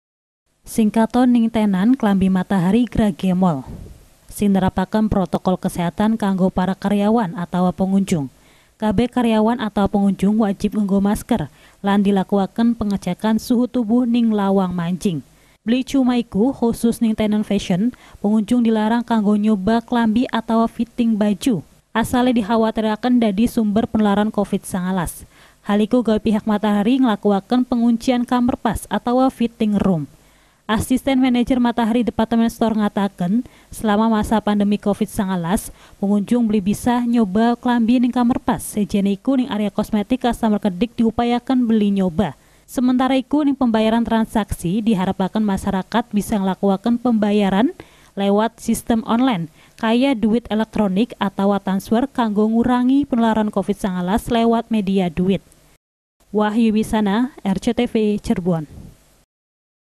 JALANKAN PROTOKOL KESEHATAN
SEMENTARA IKU / NING PENERAPAN PROTOKOL KESEHATAN / MASYARAKAT SING TEKA MENG TENANT FASHION DILARANG KANGGO NGECOBA KLAMBI ATAWA FITTING KLAMBI NING KAMAR PAS // SEJENE IKU / KABEH AREA PINTU MANJING SAMPE KASIR WIS DISEDIAAKEN HANDSANITIZER